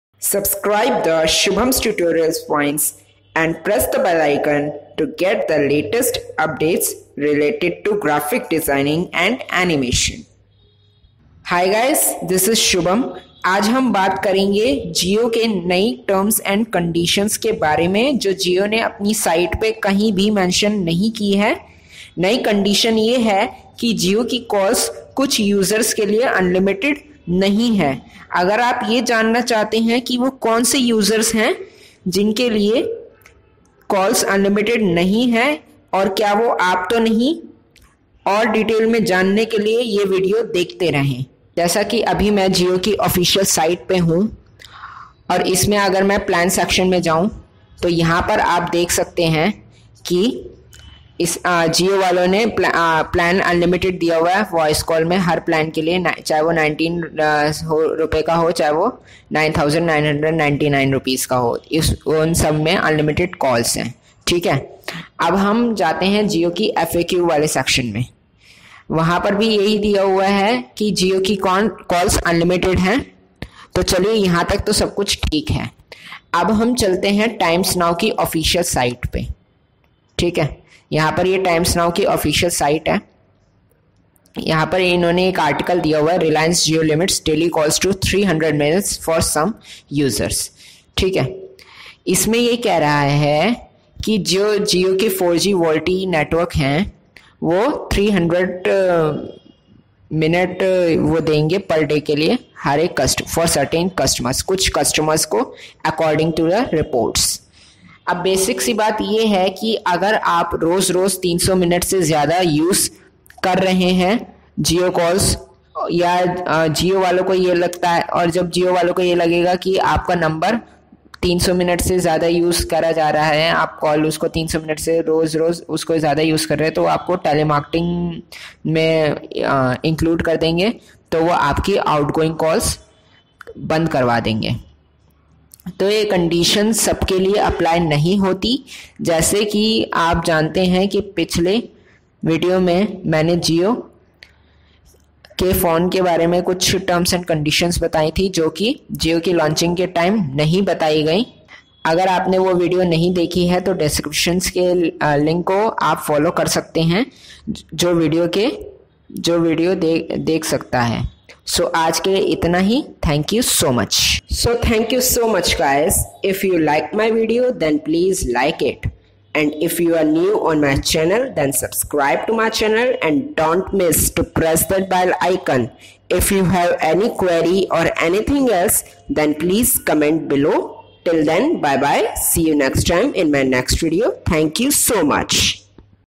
Jio 4G Calls are limited for some users|Hidden terms and Conditions| Limit 300 Min FUP on voice call
In this I will explain some hidden terms and condition or hidden Details of voice calls.Hi guys this is SHUBHAM and welcome to Shubham's Tutorialspoint.

to my channel and don't miss to press that bell icon.